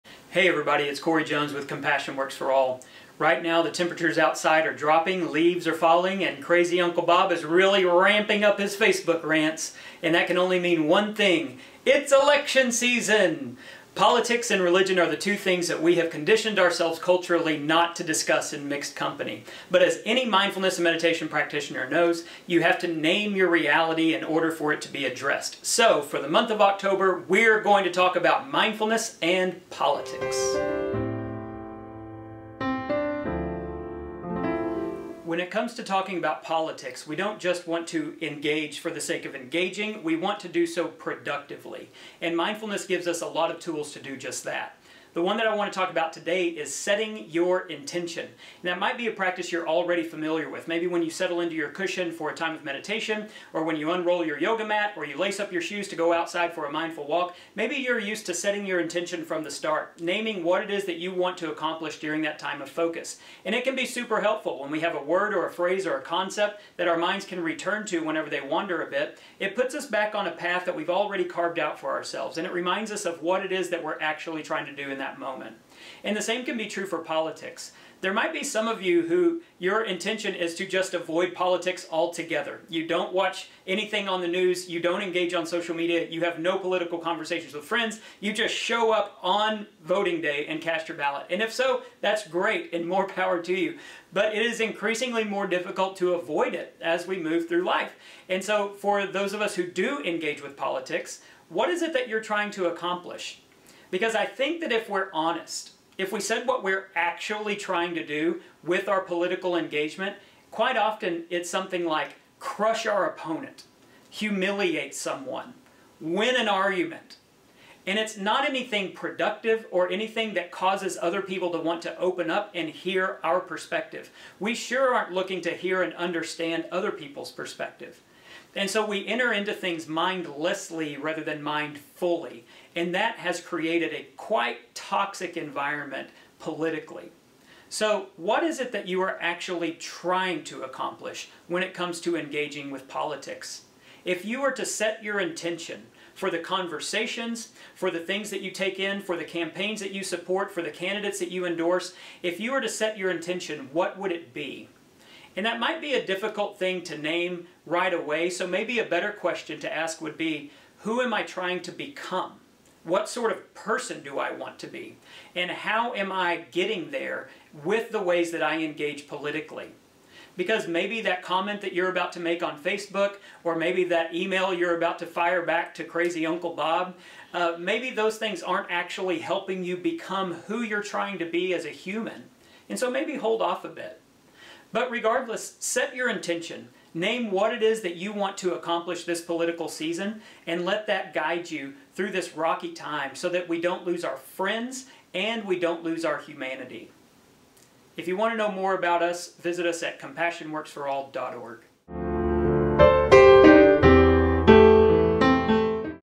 Productive Politics : Setting your Intention
In this video Compassion Works For All, director Cory explains the importance of setting our intentions. It's election season and it can be a stressful time to deal with different political views from our friends and family but it's important to be active in our government. We have to ask ourselves what our intentions are and what we want to accomplish when we're talking about politics.